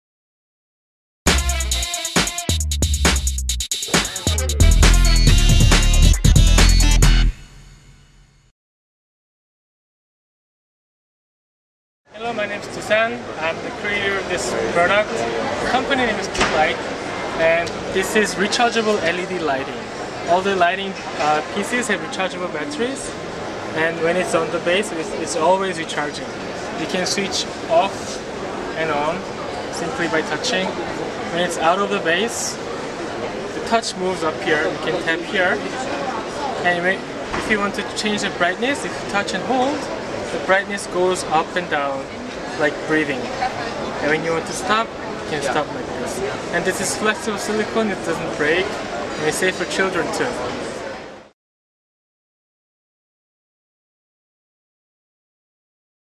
discover Doo light
buymedesign team has spotted Doo light, drop light mini design by Doosan baek from Korea.
One of our favourite idea for this season at Maison et Objet 2012. You can turn the light on/off by simply touching the top lid of the drop. It is soft, safe and light.